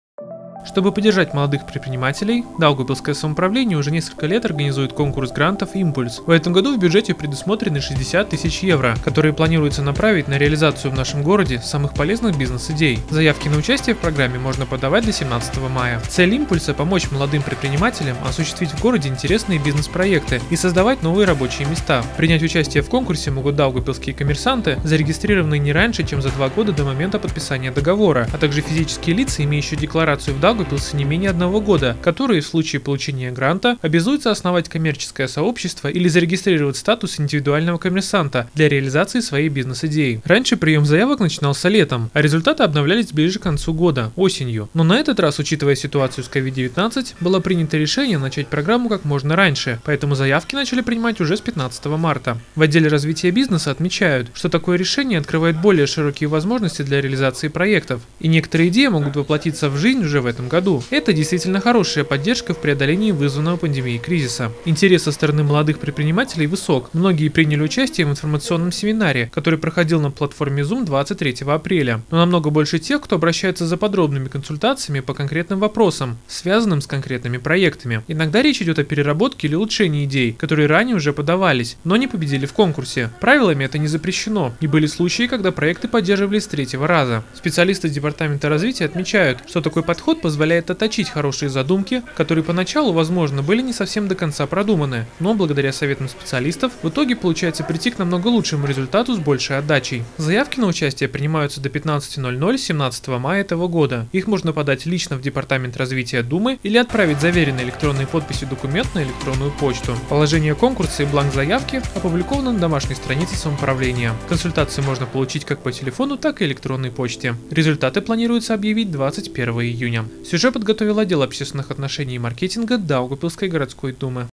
До 17 мая можно подать заявку на участие в «Импульсе»
Даугавпилсское самоуправление, чтобы поддержать молодых предпринимателей, уже несколько лет организует конкурс грантов «Импульс». В этом году в бюджете также предусмотрены 60 000 евро, которые планируется направить на реализацию в нашем городе самых полезных бизнес-идей. Заявки на участие в программе можно подавать до 17 мая.
Цель «Импульса» - помочь молодым предпринимателям осуществить в городе интересные бизнес-проекты и создавать новые рабочие места.